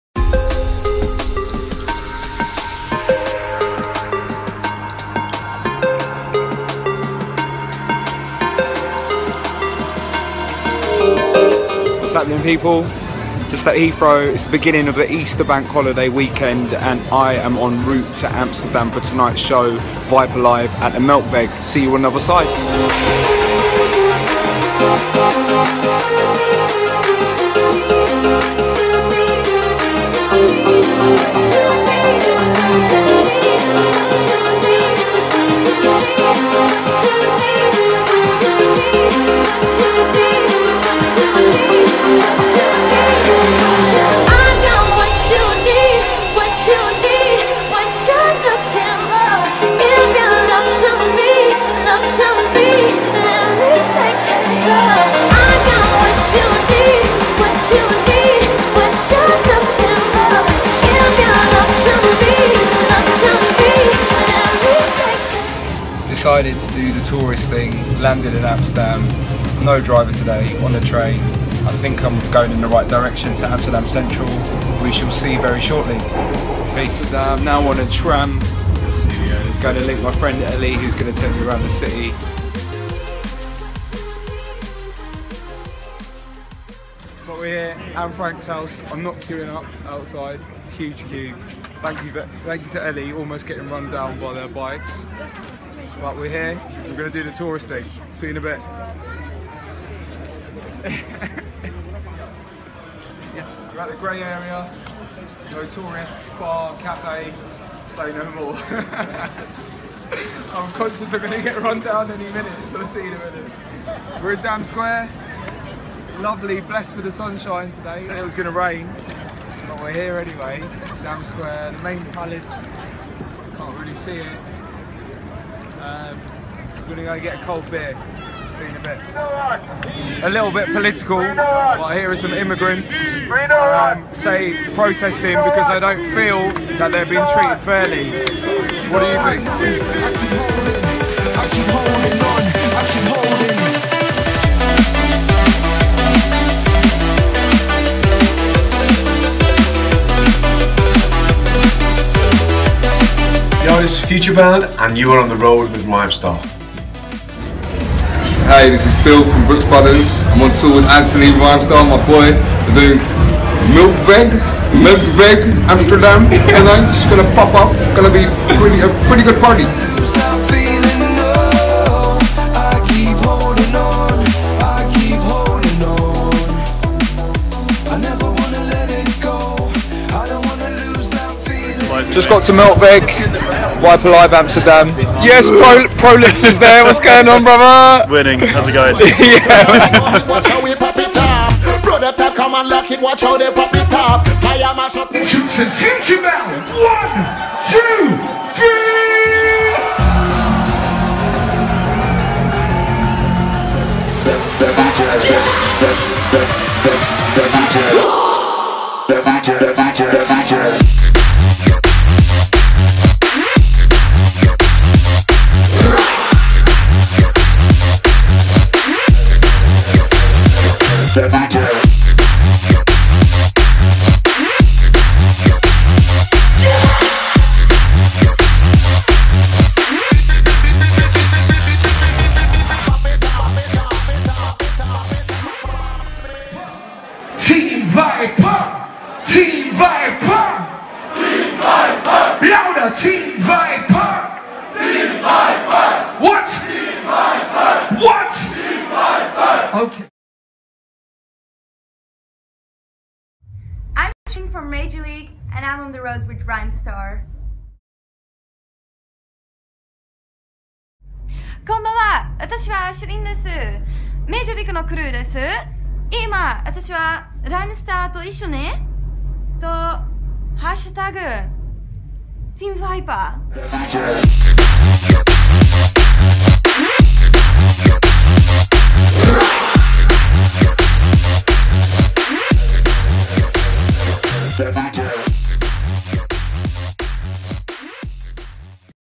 On The Road With Rhymestar - Episode 1
I travel to Amsterdam to perform at Major League Presents Viper Live, alongside Matrix & Futurebound, Brookes Brothers and Prolix